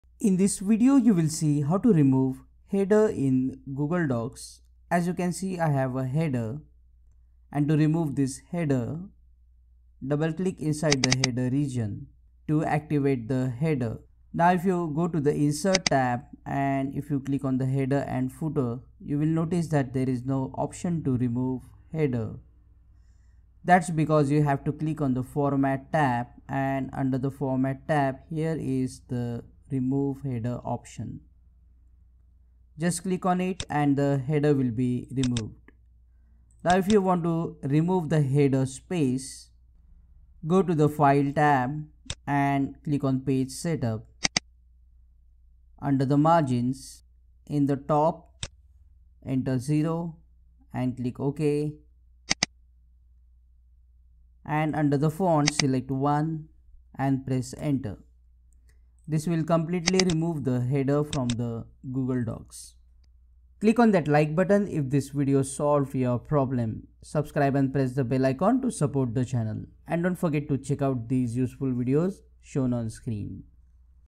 How To Remove Header In Google Docs - [ New Update ]
Watch in this video How To Remove Header In Google Docs and remove header space